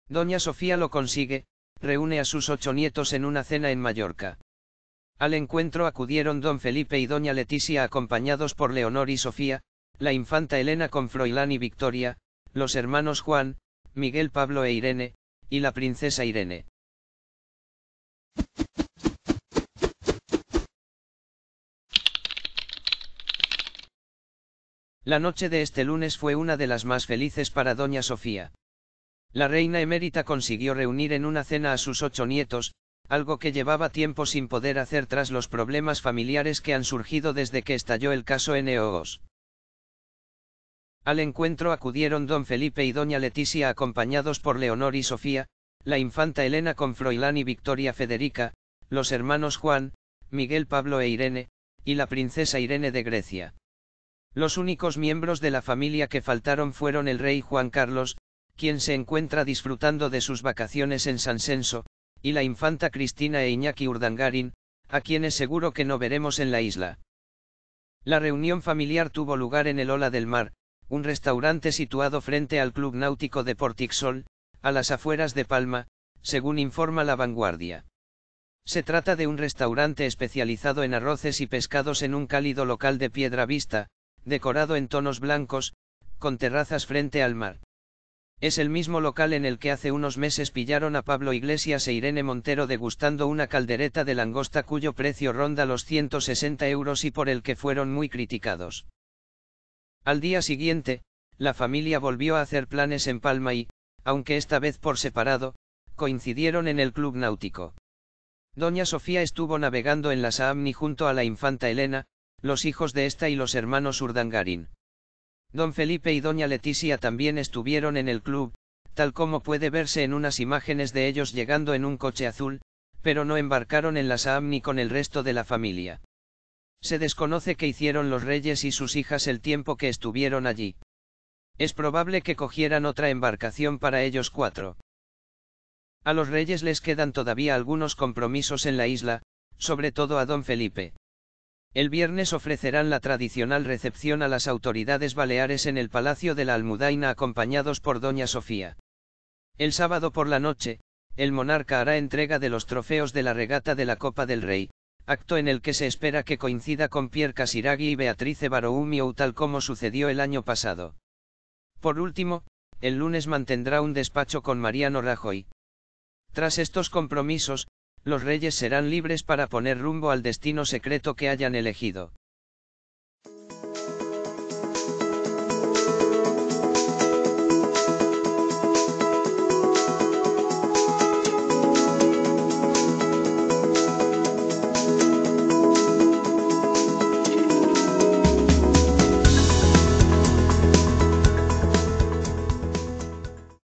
Doña Sofía lo consigue: reúne a sus ocho nietos en una cena en Mallorca
★ Summary ➜

Al encuentro acudieron don Felipe y doña Letizia acompañados por Leonor y Sofía, la infanta Elena con Froilán y Victoria, los hermanos Juan, Miguel Pablo e Irene, y la princesa Irene